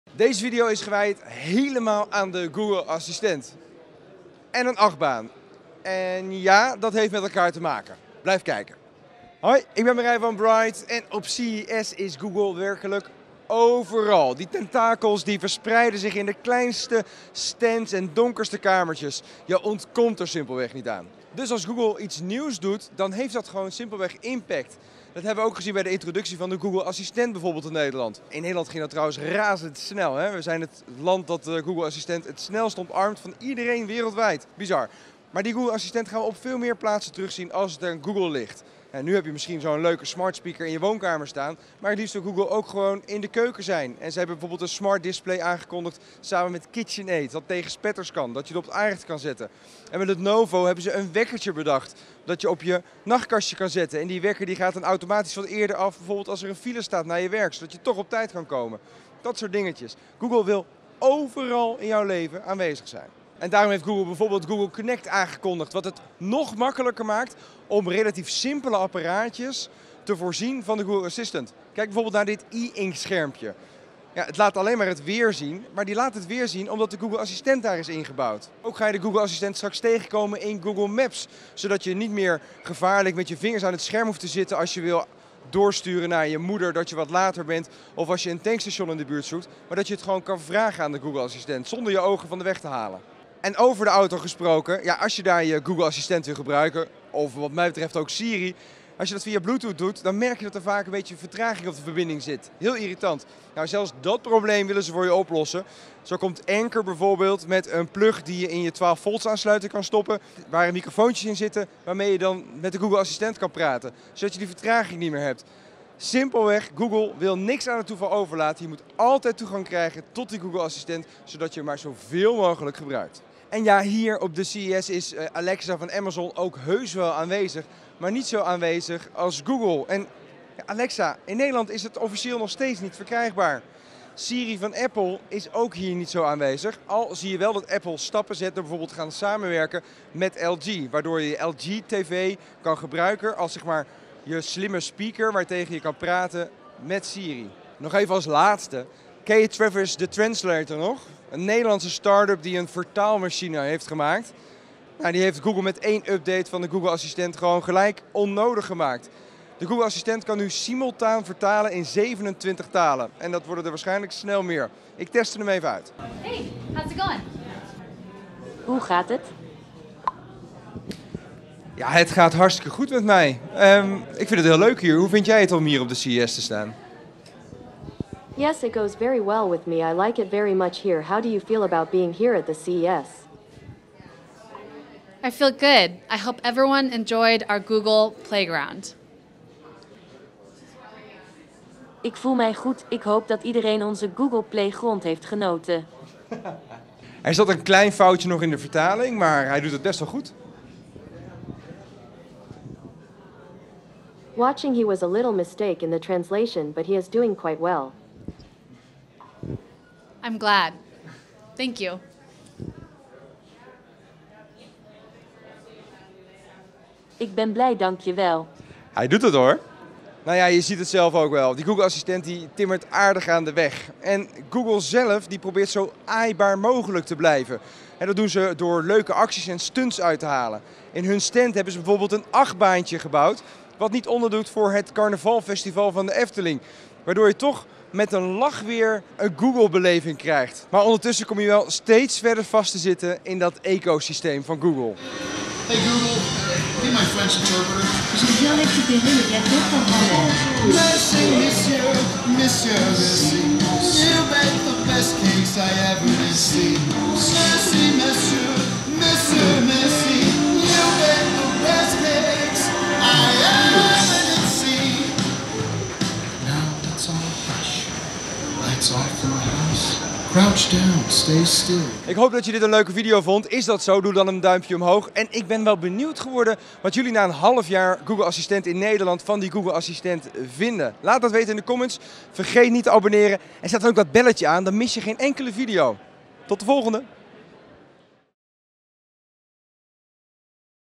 Geen ontsnappen mogelijk: Google Assistent is nu overal
De Google Assistent is binnenkort op nog veel meer apparaten te gebruiken. Wat is er nieuw aan de spraakassistent? En hoe goed werkt de vertaalfunctie? 🙏Geef een 👍 en zet de 🛎 aan
REAGEER: Waarvoor gebruik jij een spraakassistent?